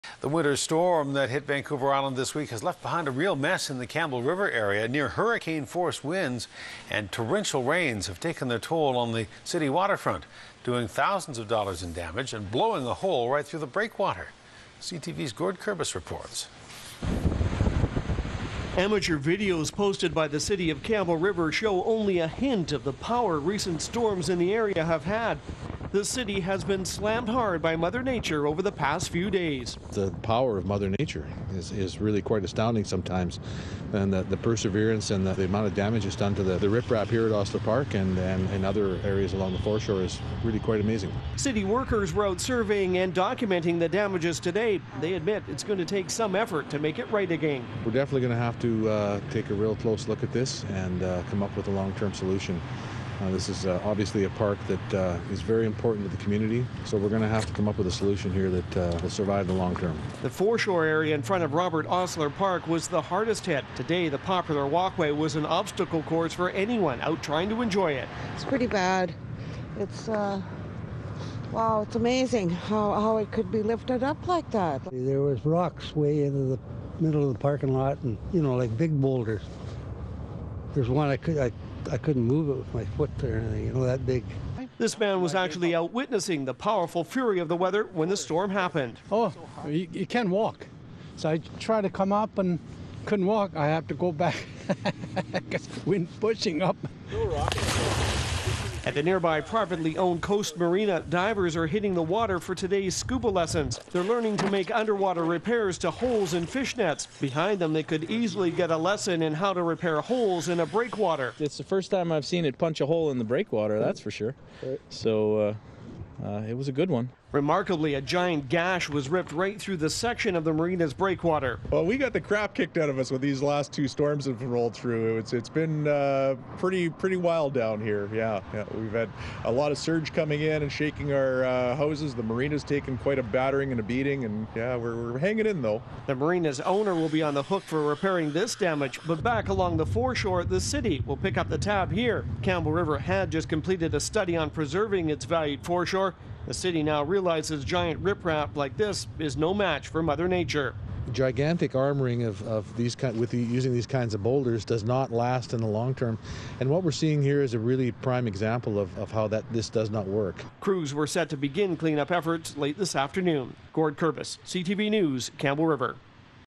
Campbell River Facing Cleanup Bills After Storms Slam Foreshore
CAMPBELL RIVER - The winter storm that hit Vancouver Island this week has left a real mess in the Campbell River area.

Near hurricane force winds and heavy rains have taken their toll on the city's waterfront, doing thousands of dollars in damage and blowing a hole right through the breakwater.